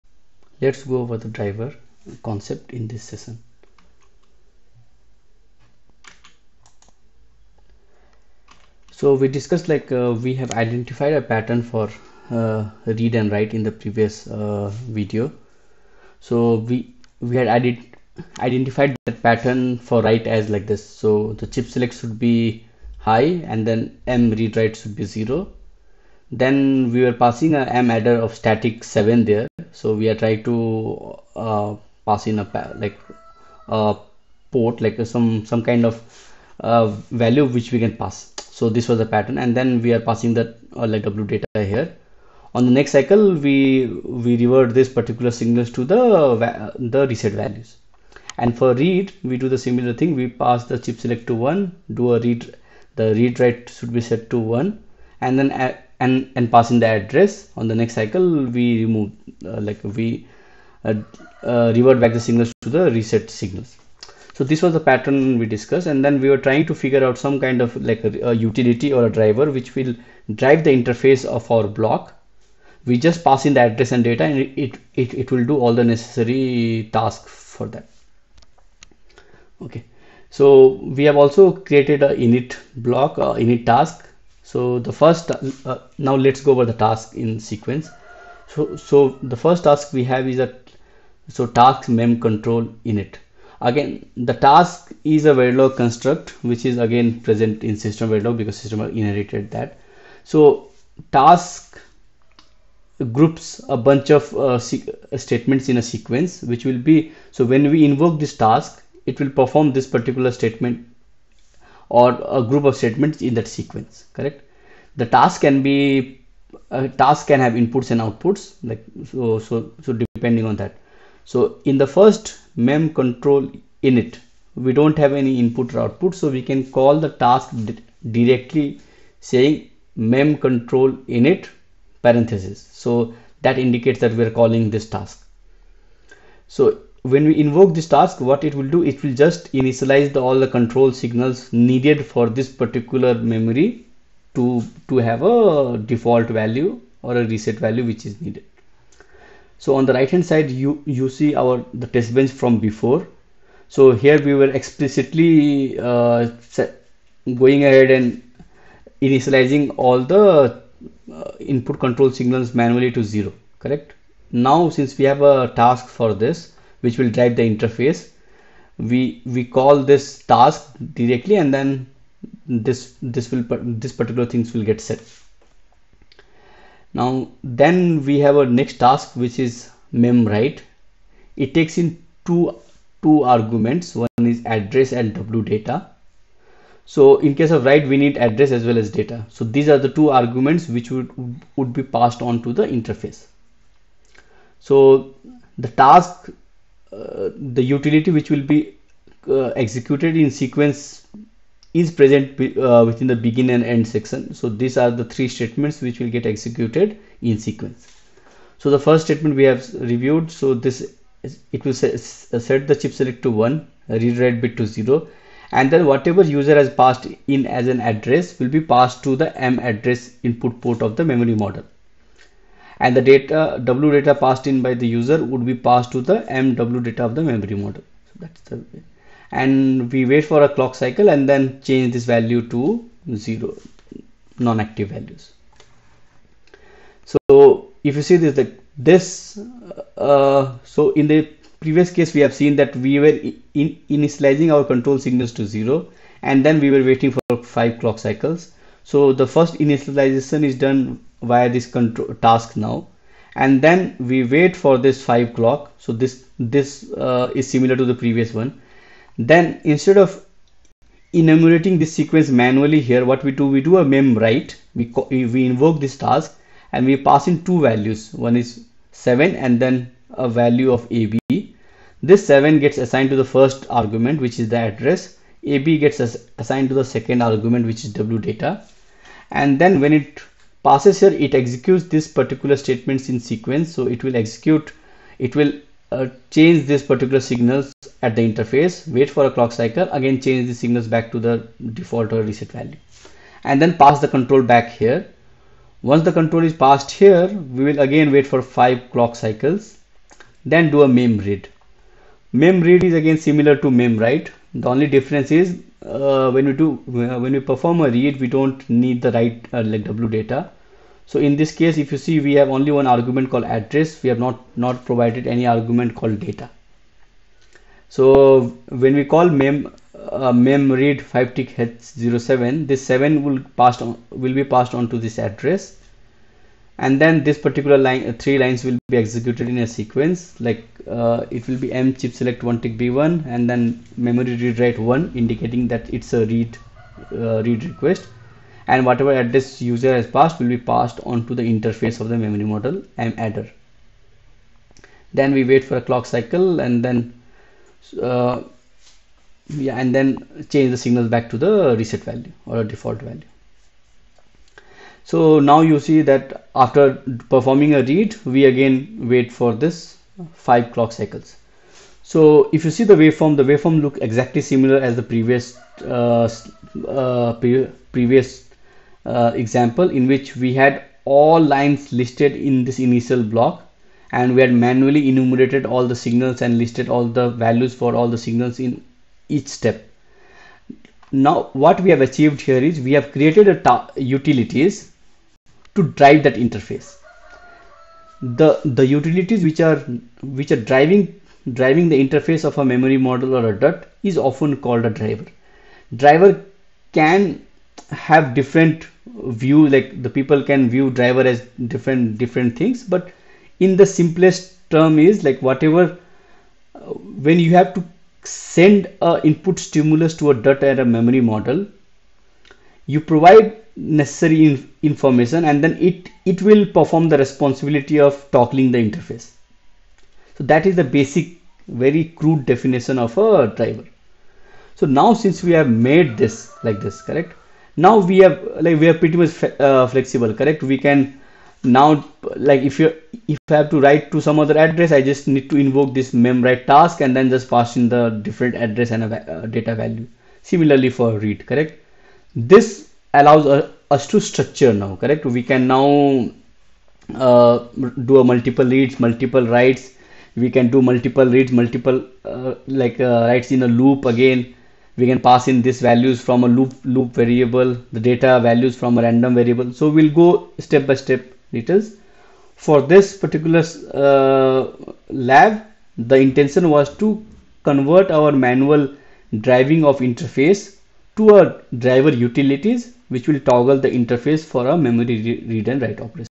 Driver - Quick Verilog Review :: Part 1 Verification Concepts :: SystemVerilog - Verification
This video will show how we can update manual driving of interface into driver utilities which provides cleaner as well as better approach for verification.